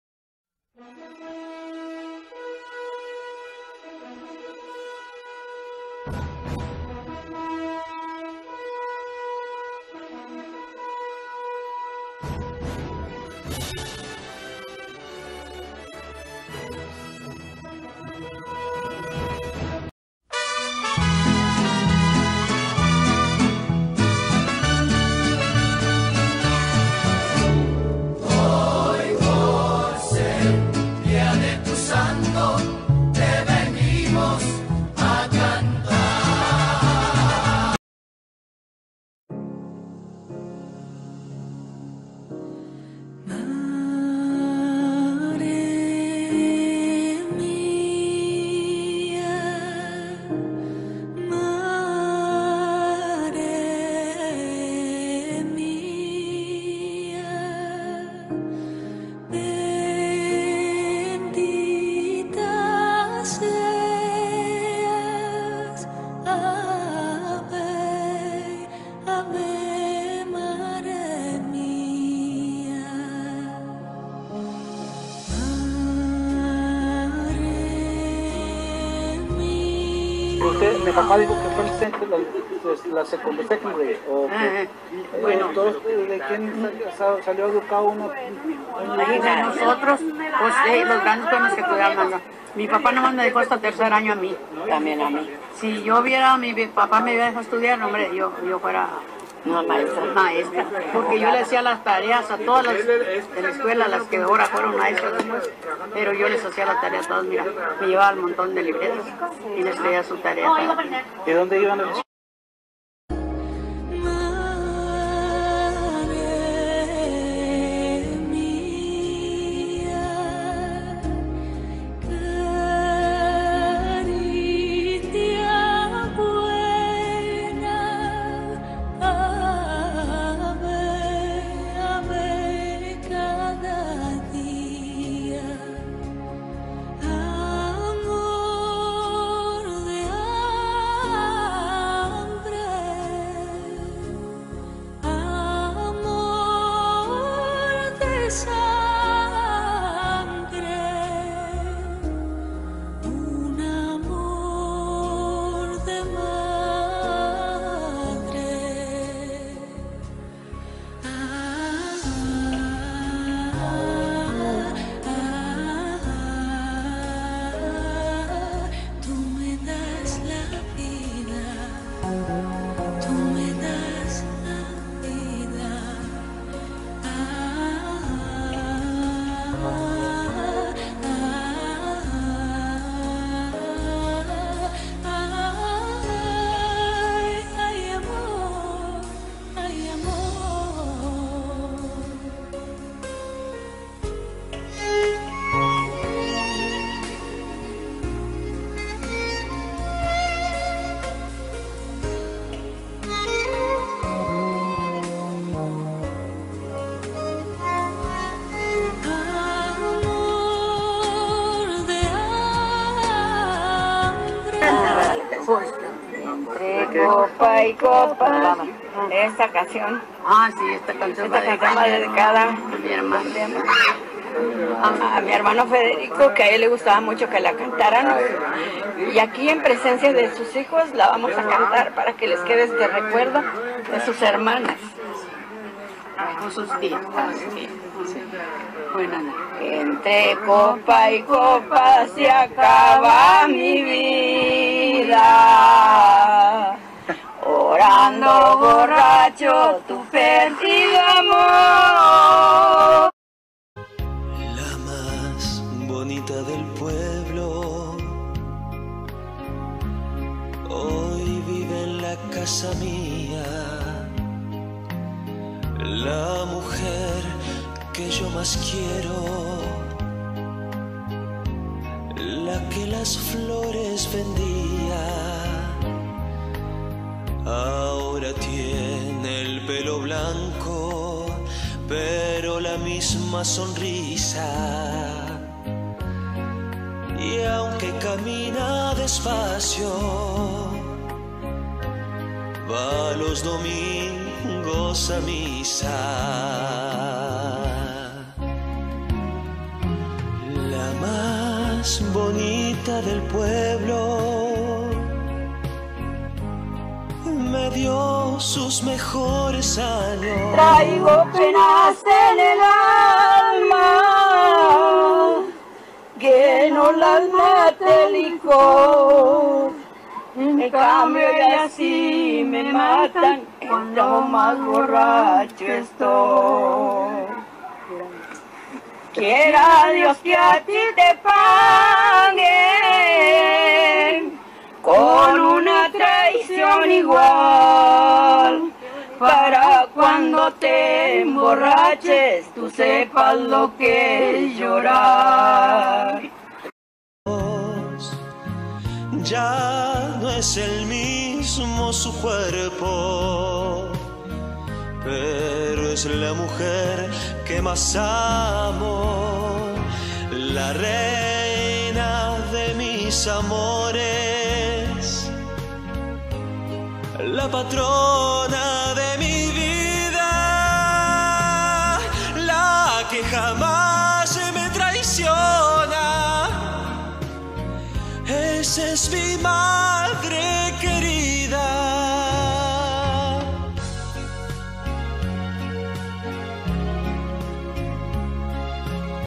VIDEO PARA MI MAMÁ EN SU CUMPLE AÑO 2014
ESTE VIDEO SE LO HICE CON MUCHO CARIÑO A MI MADRE POR SU CUMPLEAÑOS ESTE 5 DE OCTUBRE DEL 2014...